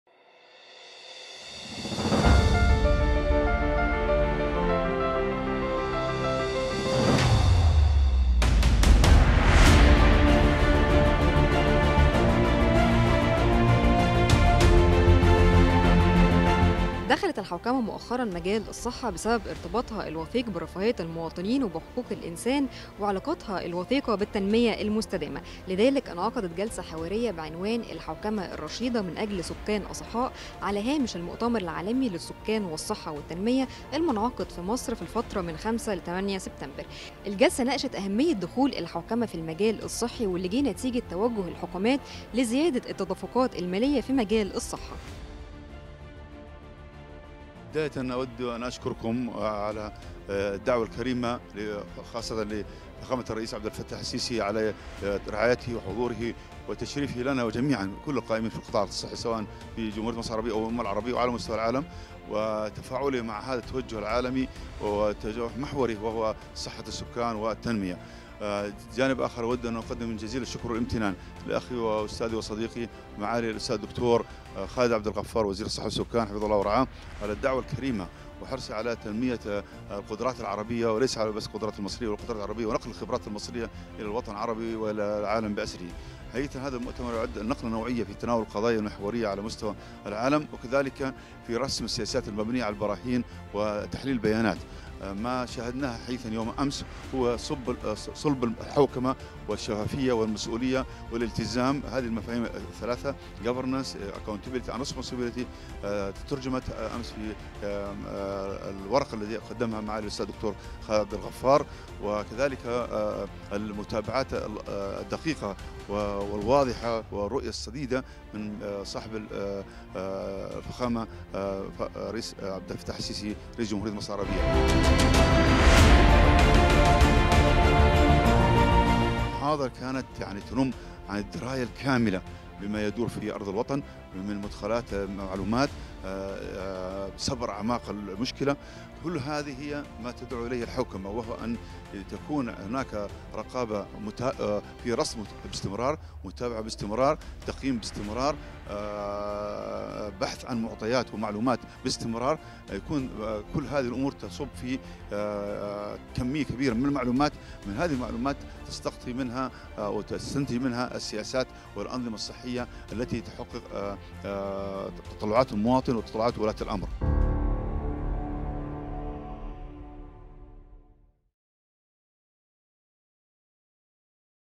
عقد جلسة حوارية بعنوان "الحوكمة الرشيدة من أجل سكان أصحاء"
ضمن فعاليات المؤتمر العالمي للسكان والصحة والتنمية.. عقد جلسة حوارية بعنوان "الحوكمة الرشيدة من أجل سكان أصحاء"